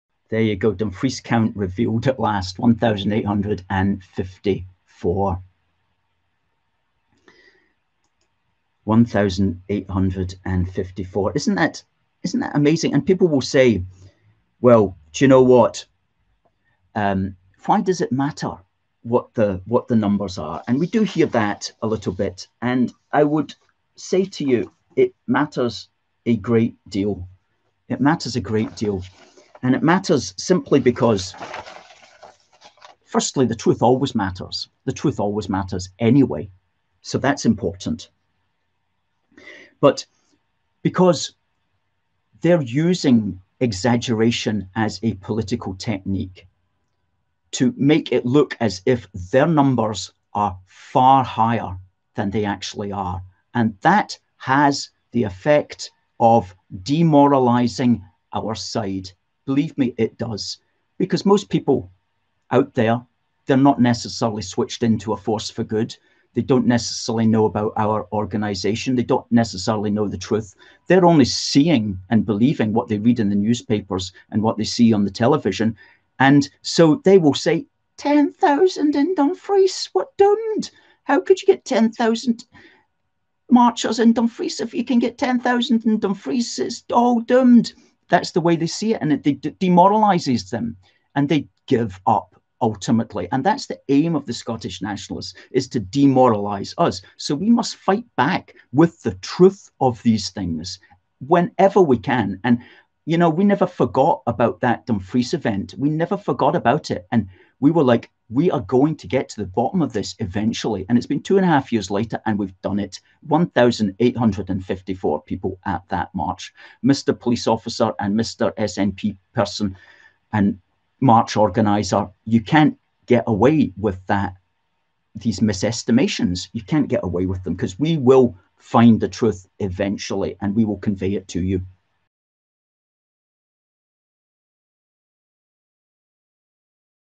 We Prove ScotNats Lie about Marches
Scottish Unionist pro-UK group A Force For Good says that the Truth always matters and it is important that we do not allow the Scottish nationalists to demoralise us with their exaggerations and lies! We must fight back with the Truth!

Alistair McConnachie, Director at AFFG, reveals that there were not "10,000" at the SNP-supporting march in Dumfries on 2 June 2018. Using video evidence, we have counted them at 1,854, as we point out in our article at